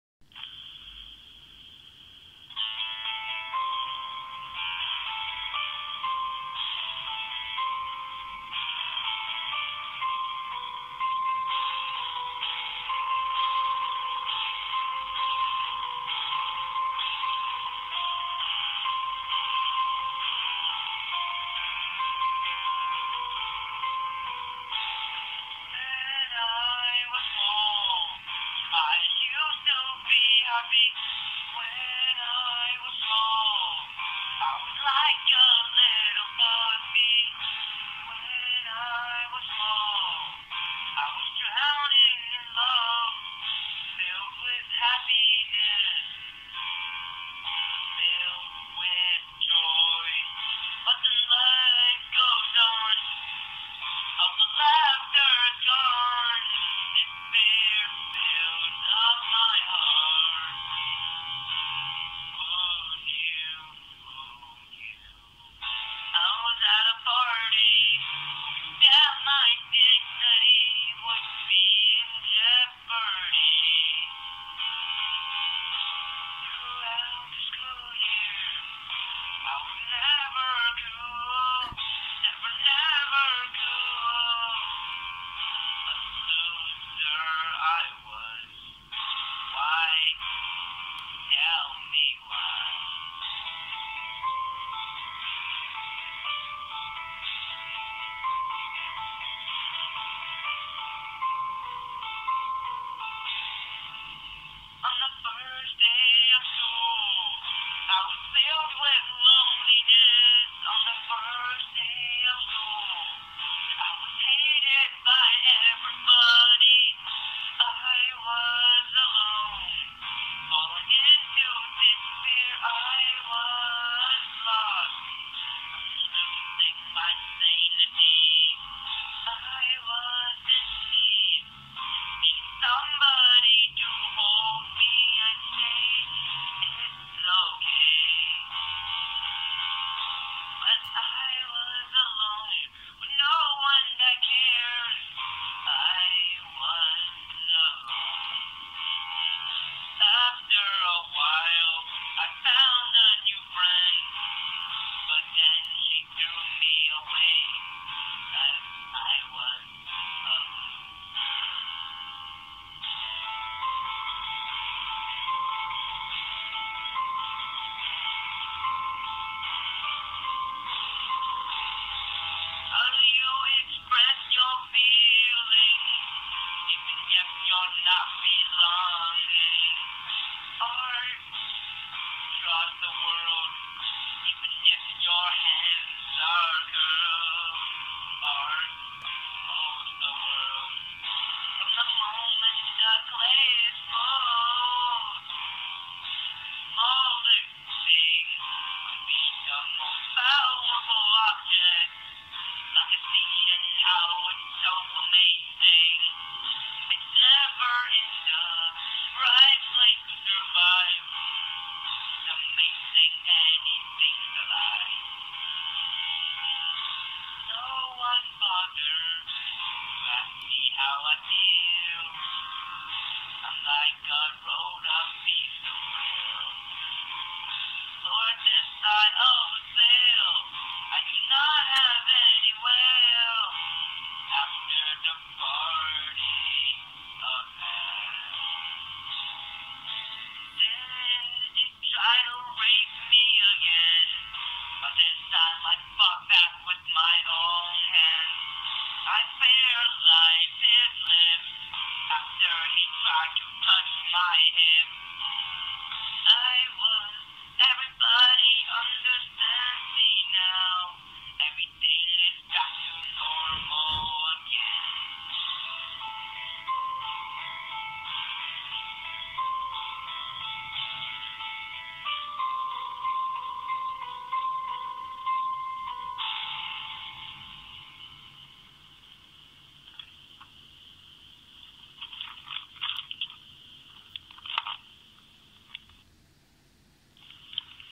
2011/10/13
I wrote and sung this for a grade 9 English project and feel I should of gotten better than a B